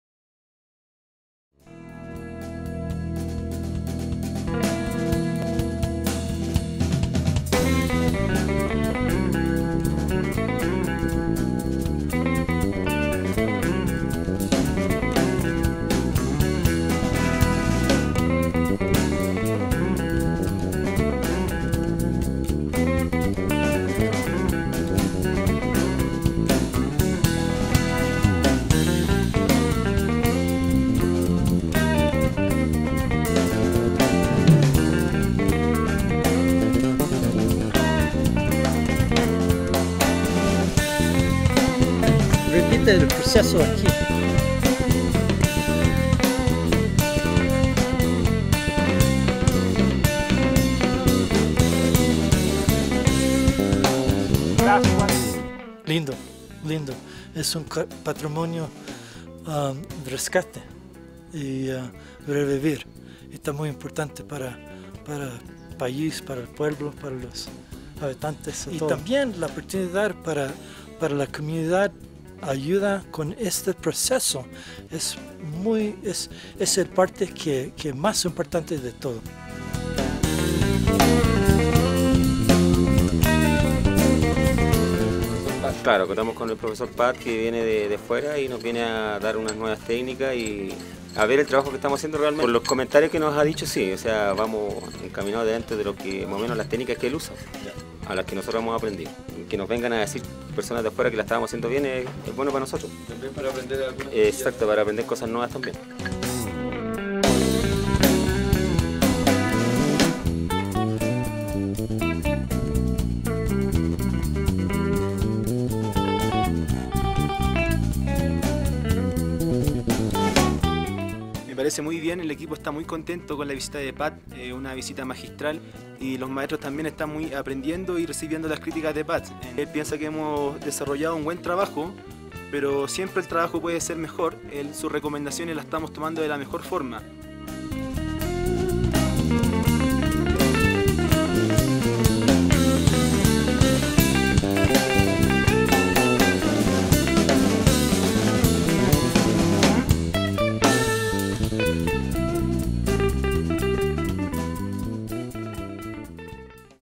Visita obras Febrero 2014 | Fundación Altiplano
Visita obras de FAMSV Febrero 2014, con invitados ilustres:
El ingeniero Julio Vargas de Perú y don Pat Taylor, directamente desde Nuevo México, USA.